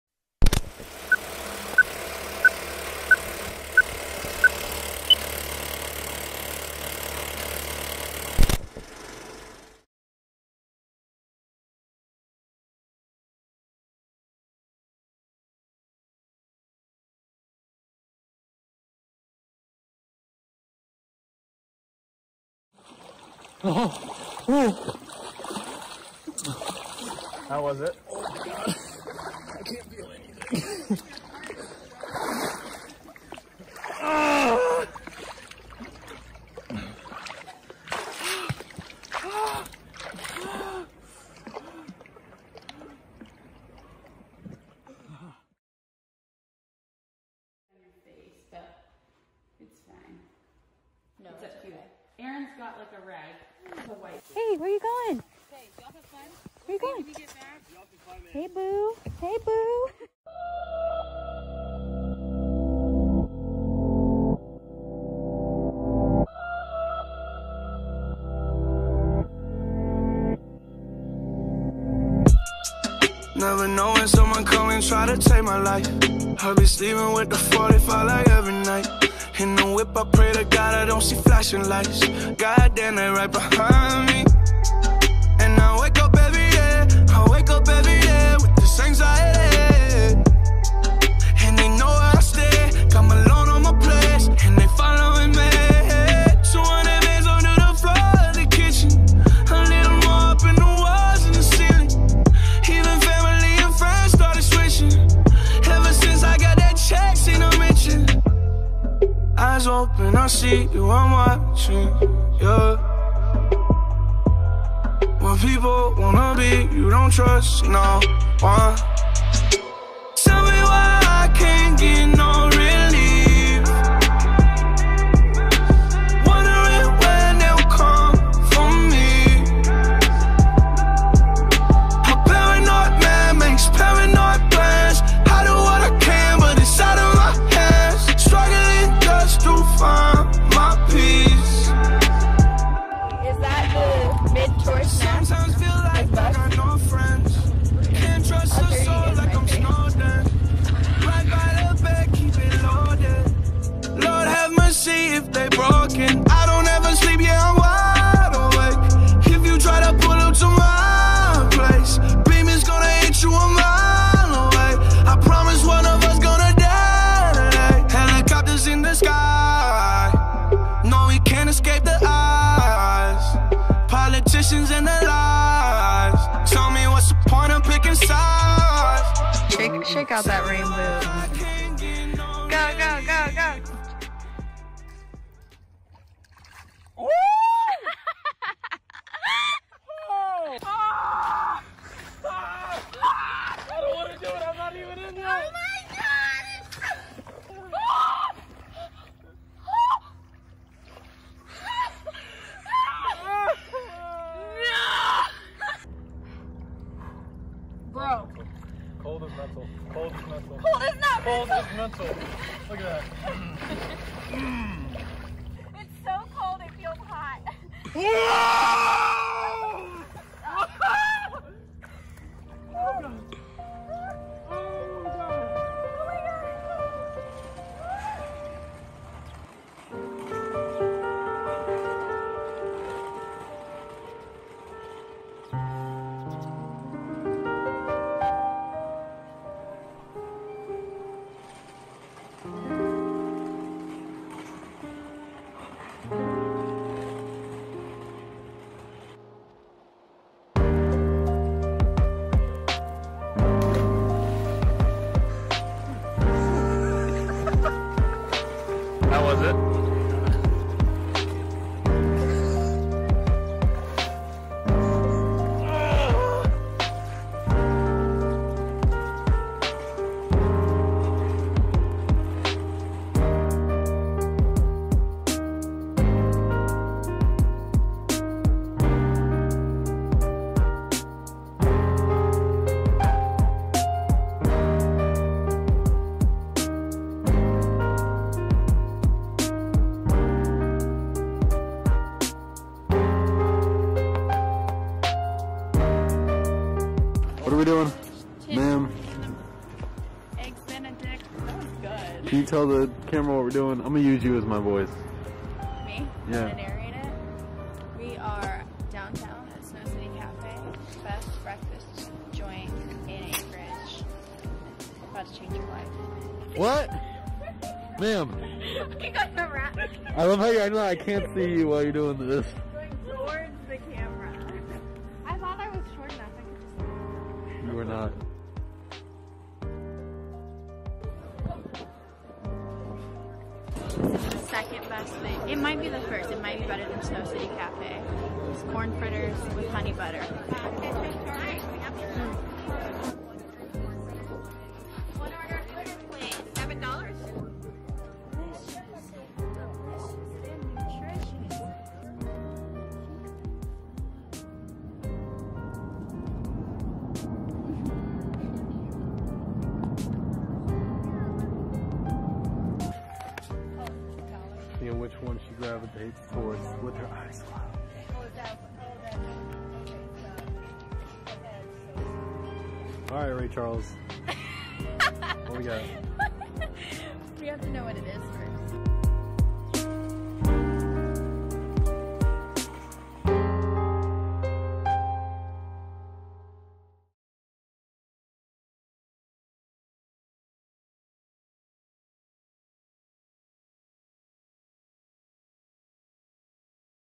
Alaska ATV Glacier Adventure
Takes a truly wise soul to think swimming in a freezing cold Alaskan Glacier is a good idea, right?

We booked this trip Through Alaska Backcountry Adventure Tours and had an absolute blast!